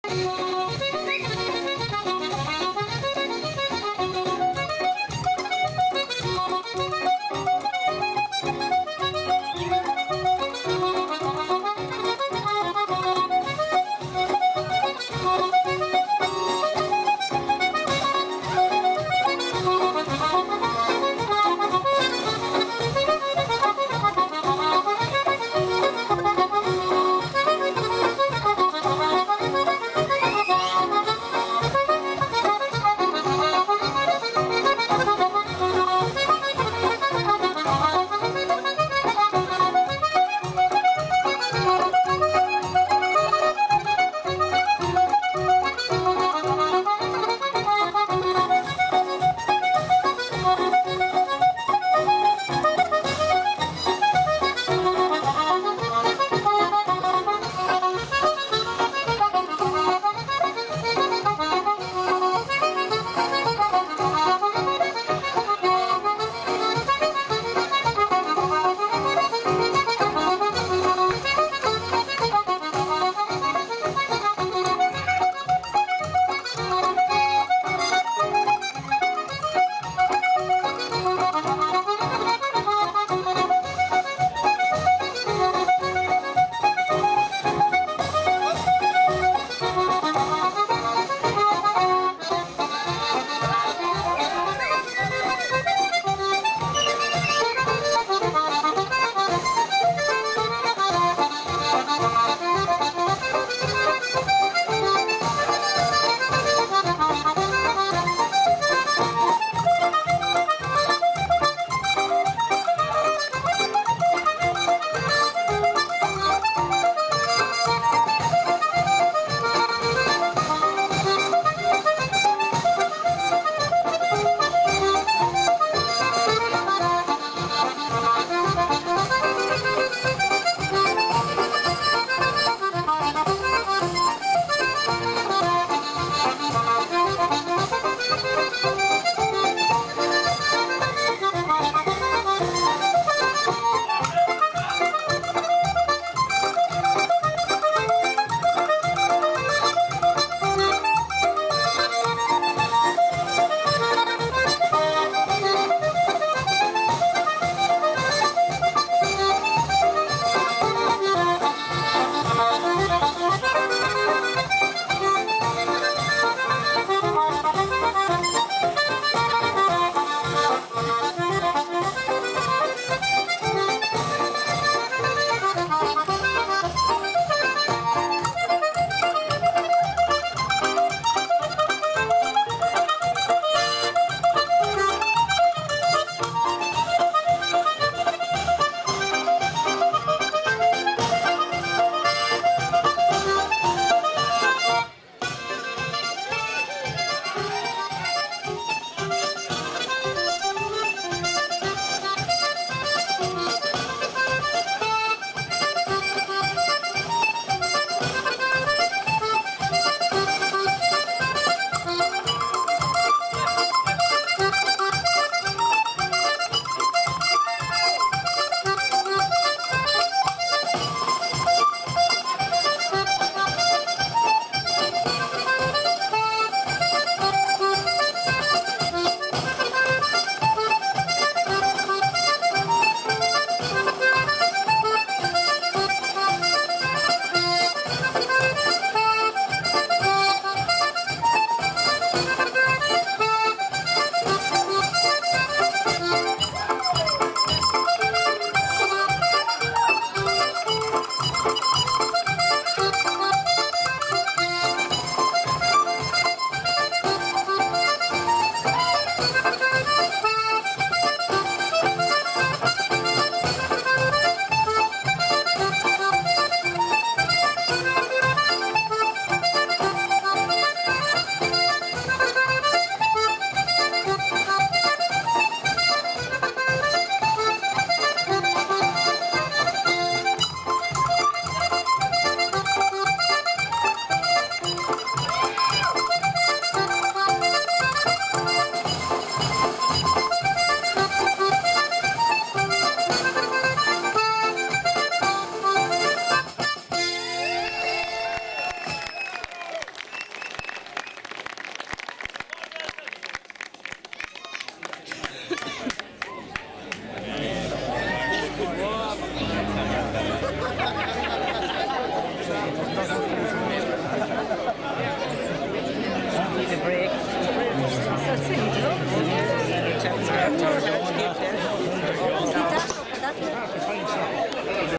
Darren Breslin
Great box playing from Darren at the Westmeath Fleadh Cheoil, in Ballynacargy, with Moyra Fraser and Martin Murphy, May 2013.